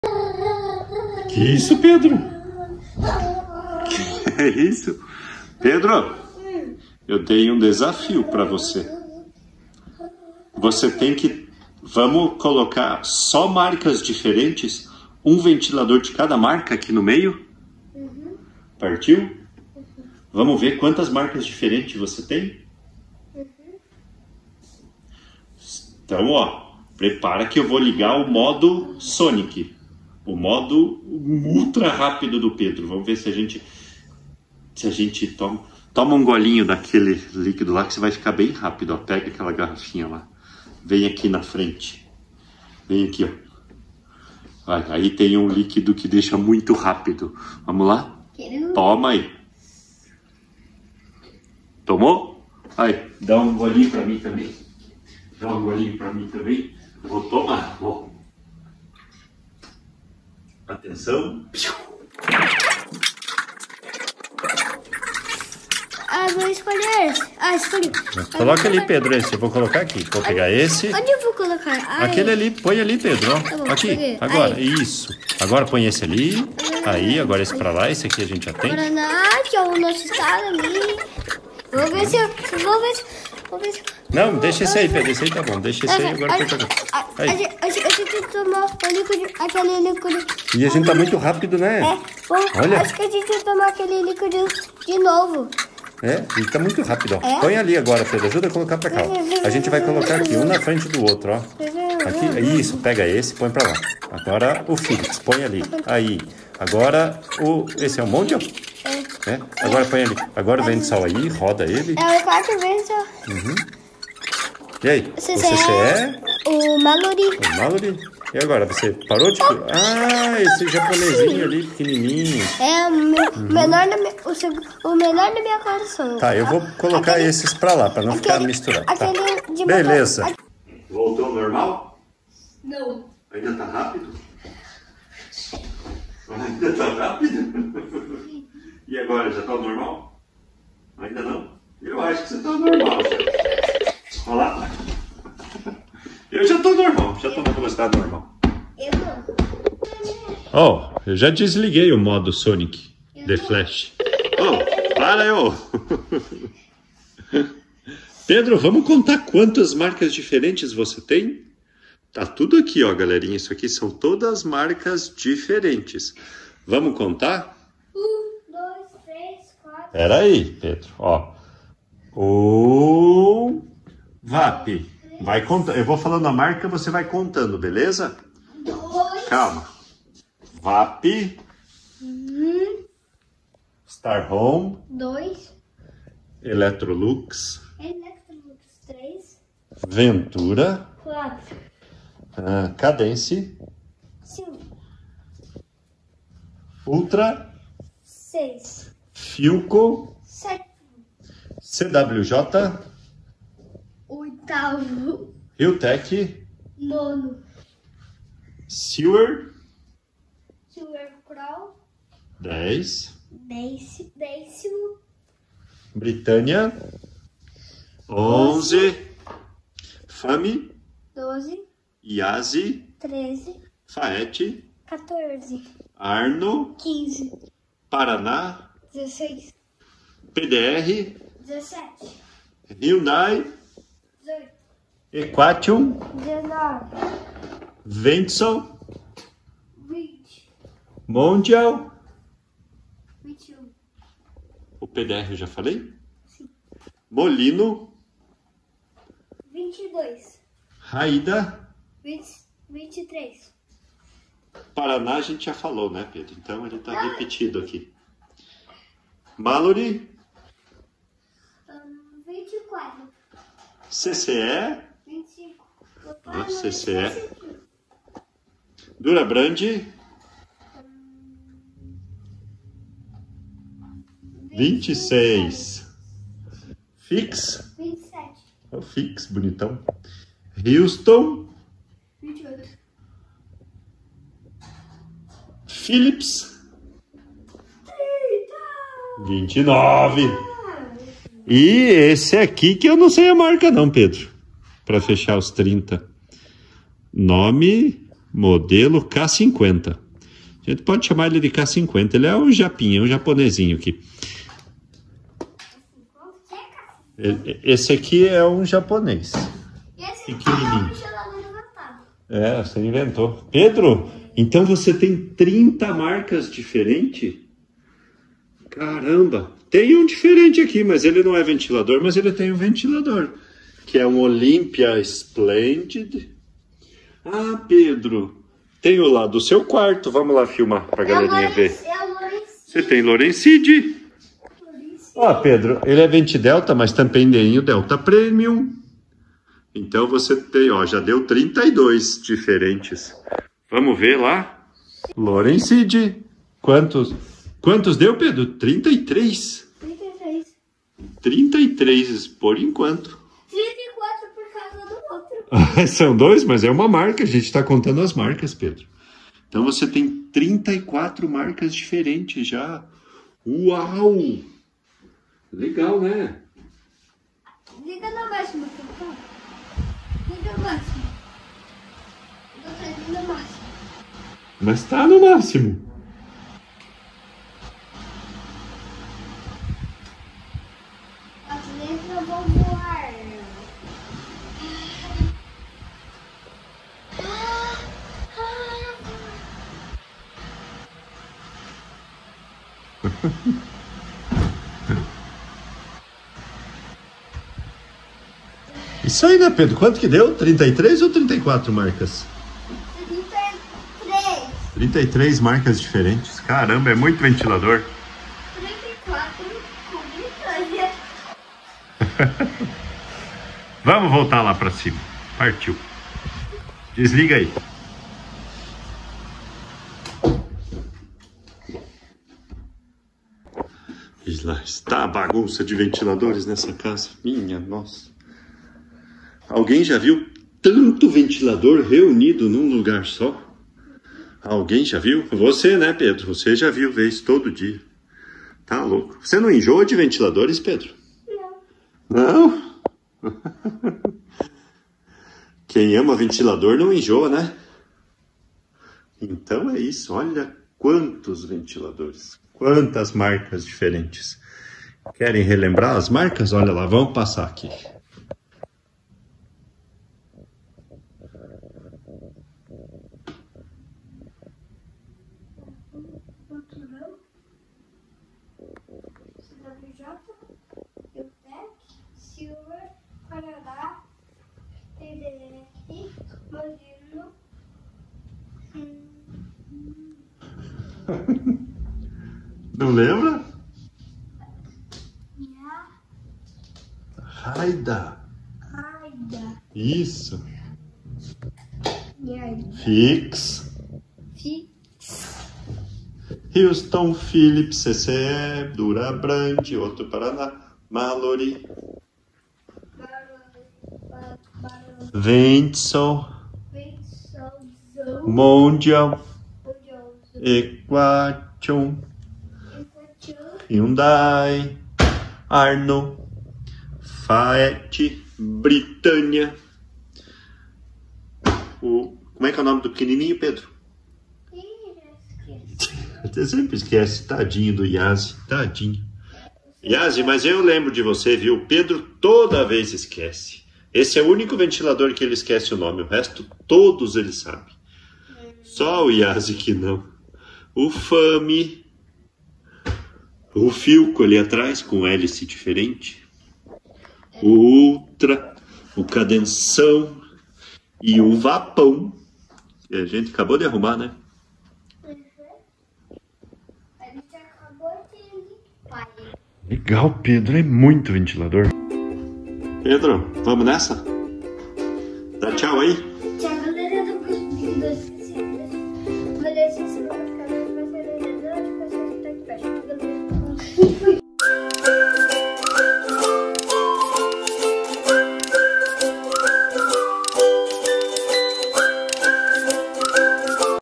Tenho mais de 30 marcas diferentes de ventiladores na minha coleção
Oi galerinha
Separamos os ventiladores pra ver quantas marcas eu tenho.
E deu mais de 30!!!